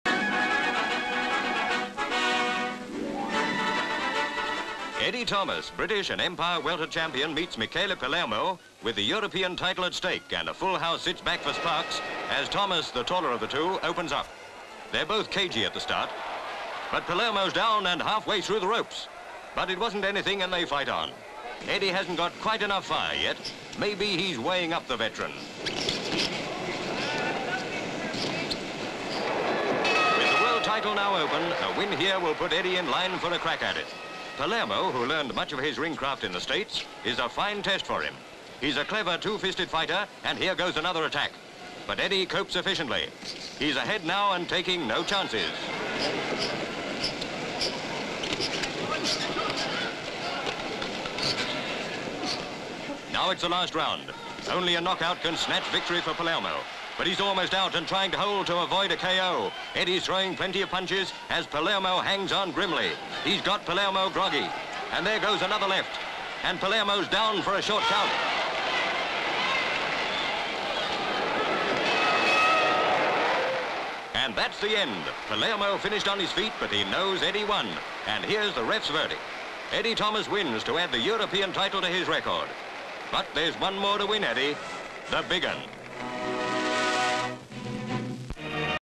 Thomas Wins Third Title (1951)
Carmarthen, Wales.

SV. Michele Palermo, European Welterweight Champion, walking across ring to shake hands with Eddie Thomas, British and Empire Welterweight Champion at start of European title boxing fight in Carmarthen, Wales. SV. Crowd. LV. Start of fight. Thomas on right, attacks. Palermo goes down. SV. Palermo getting up from floor. MV. Cautions sparring until Palermo across ring. SV. Palermo rushing across ring to pin Thomas against roped. SV. Boxers sparring against ropes, pan to crowd. SV. Referee making score card. SV. Thomas receiving attention from seconds. SV. Palermo leaving corner and sparring up. MV. Palermo sails into Thomas, who coolly fights him off with a nice left but Palermo with wild two handed attack forces Thomas into corner. SV. After left and right from Palermo referee separates them. SV. Thomas throws left and right uppercuts. SV. Feet. MV. Thomas lands a powerful left to Palermo's body and forces off Palermo who vainly tries to hang on. Thomas misses with right to Palermo's head. Palermo still hanging on. Thomas hands left. SV. Thomas lands left to Palermo's head. Palermo gets down struggles up after short count and resumes. MV. Fight stopped. Referee holds Thomas' hand aloft and Thomas walks across ring. SCU. Eddie Thomas - new champion.

(Orig. Neg.)
 FILM ID:1439.23